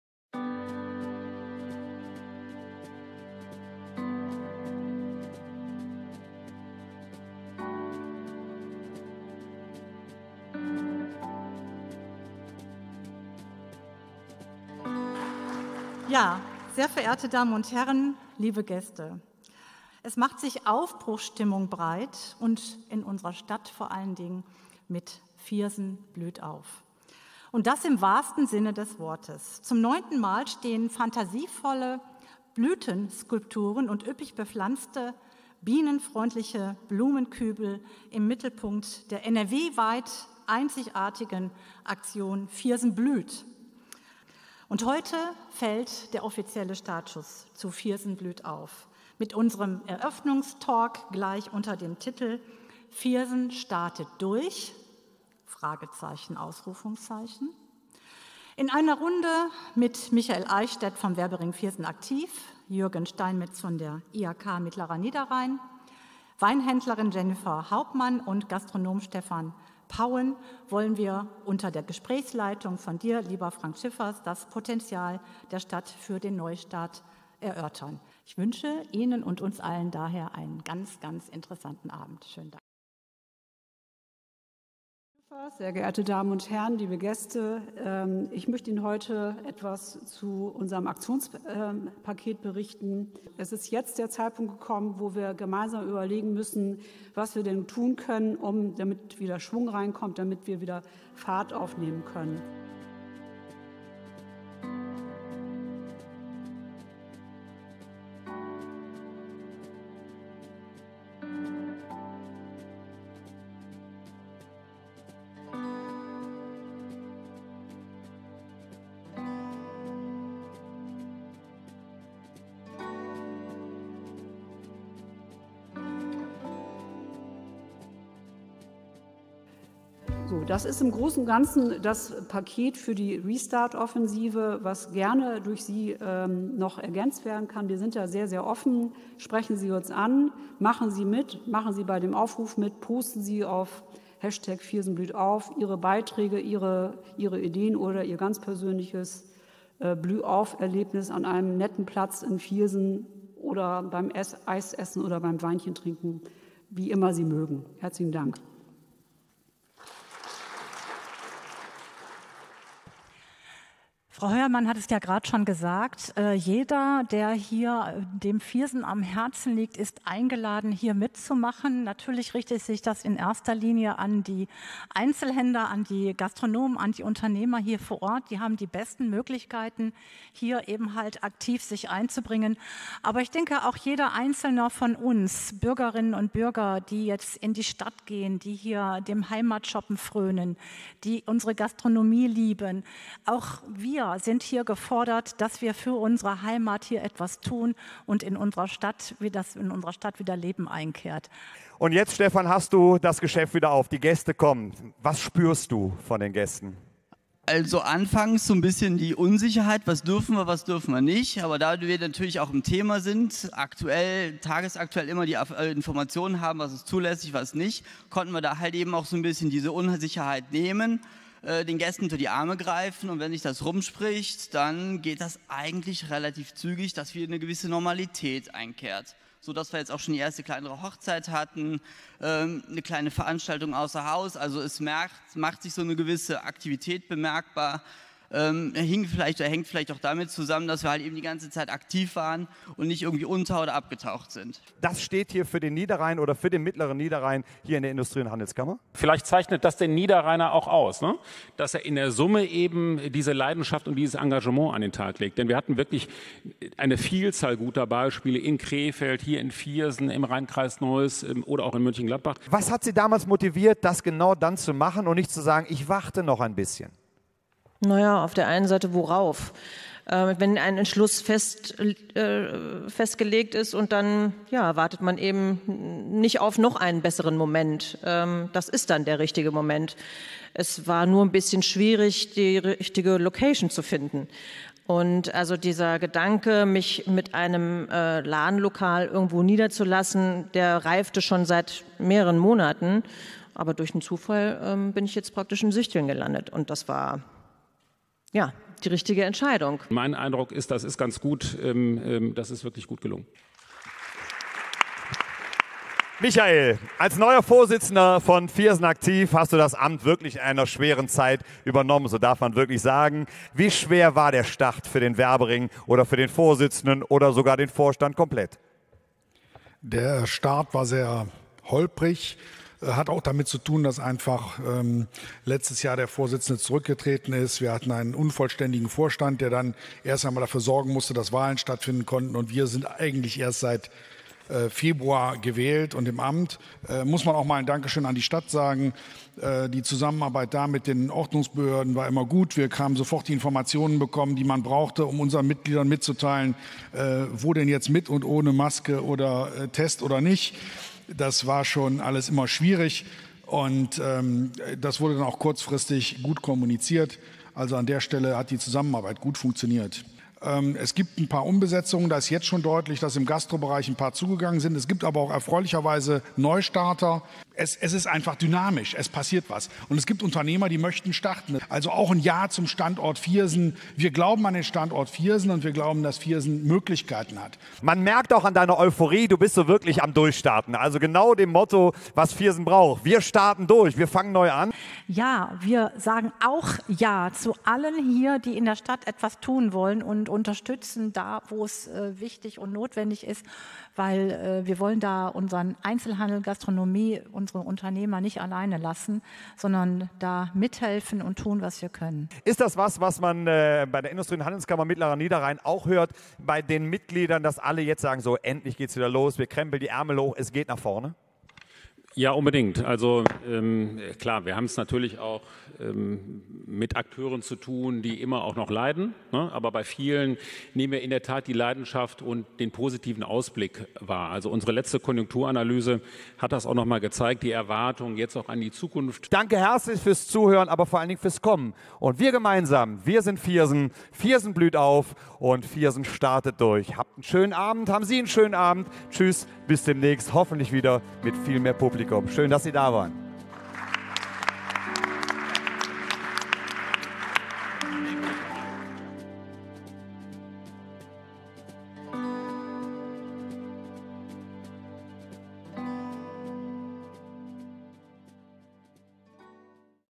Eröffnungstalk Viersenbluehtauf 2021
Den offiziellen Startschuss zu Viersenbluehtauf 2021 gab am 23. Juni 2021 der Eröffnungstalk „Viersen startet durch?!“ in der Festhalle. Die Talkrunde mit Vertreterinnen und Vertretern aus Wirtschaft und Verwaltung erörterte das Restart-Potenzial der Stadt.